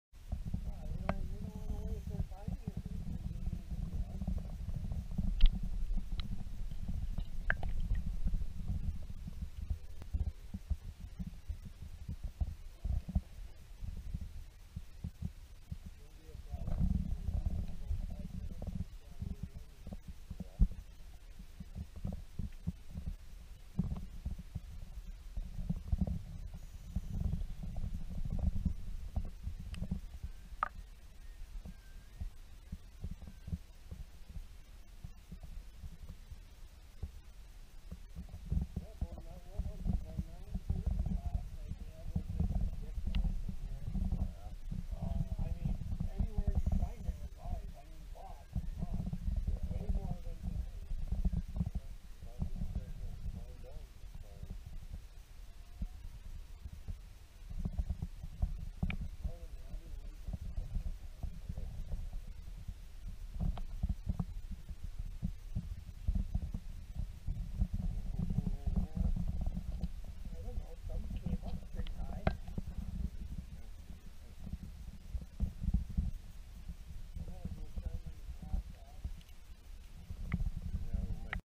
Underwater view of the Spring Walleye Run 2019
A brief view of the Spring Walleye Run in Northern Wisconsin.